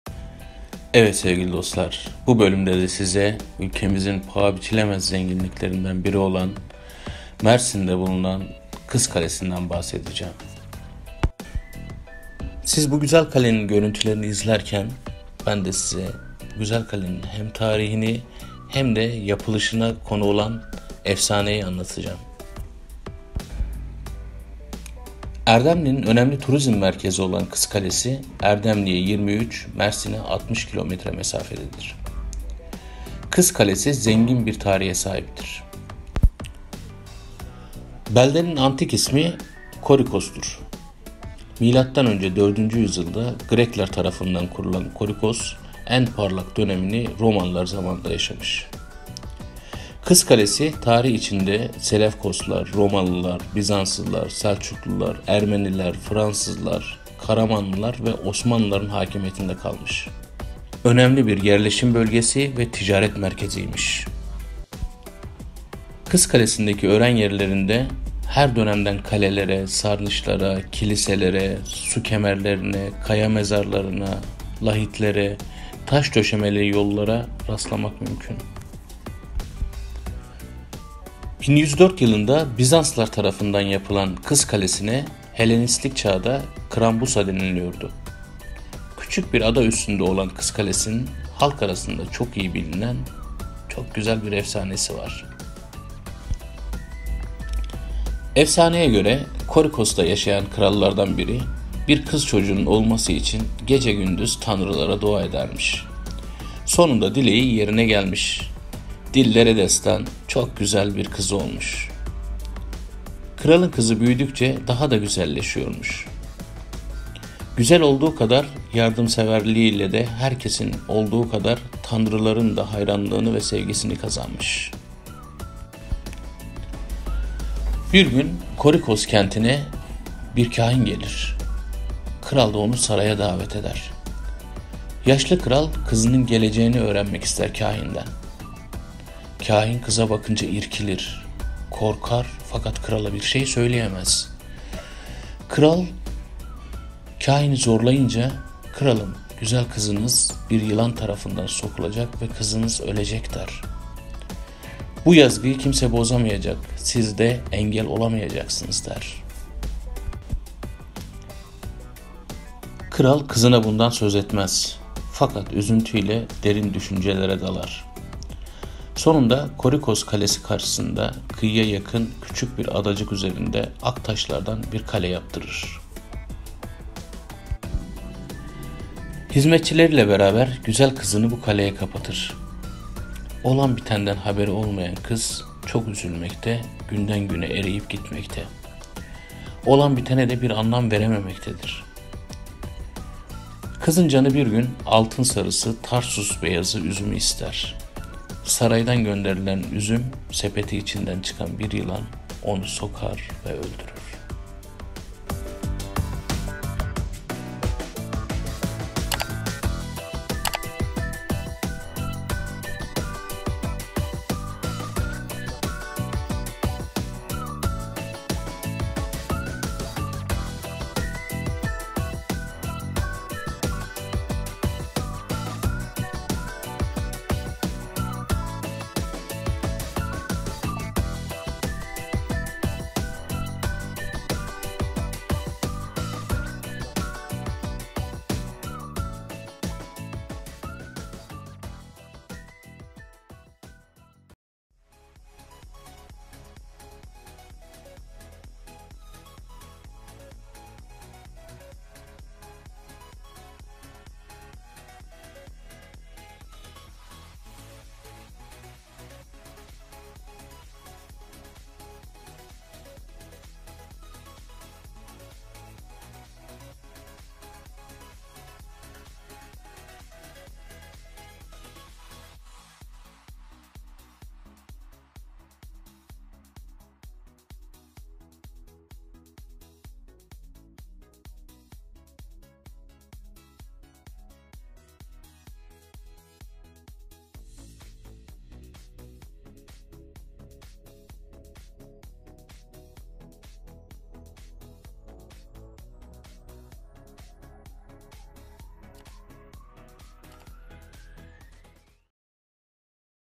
KIZ KALESİ(Mersin)EFSANESİ ve Tarihi /Original Girl Castle
Mersin'de bulunan Tarihi zenginliğimiz KIZ KALESİ'nin Tarihini ve yapılış Efsanesini anlattım.
İyi seyirler.